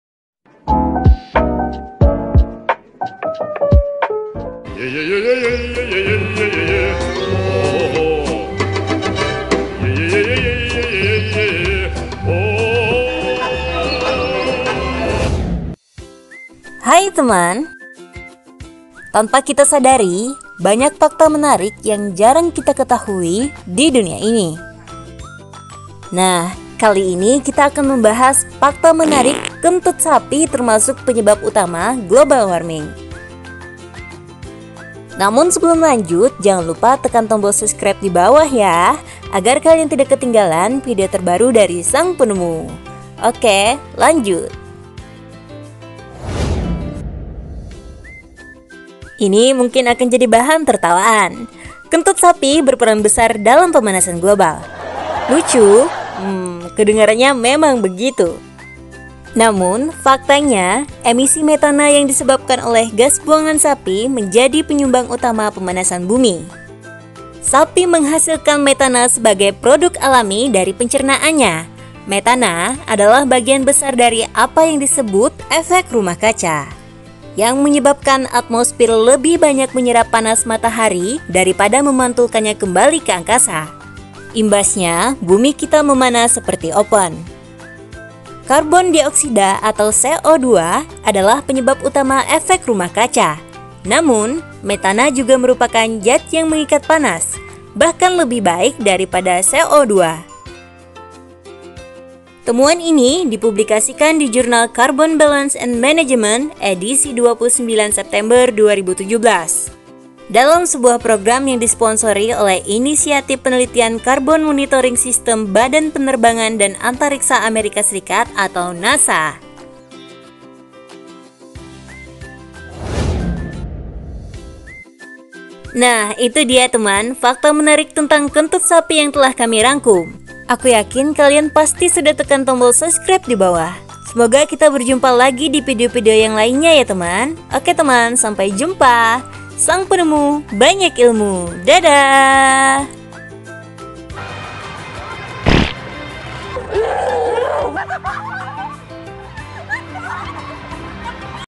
Global Warming Dari Kentut Sapi, Apakah Benar?
Fakta Unik Kentut Sapi Termasuk Penyebab Utama Global Warming

Hi, Teman !
Tanpa kita sadari banyak fakta menarik yg jarang kita ketahui di dunia ini.
Nah kali ini kita  akan membahas fakta menarik Kentut sapi termasuk penyebab utama global warming..

Okee lanjut.
ini mungkin akan jadi bahan tertawaan: kentut sapi berperan besar dalam pemanasan global. Lucu?hemss Kedengarannya memang begitu.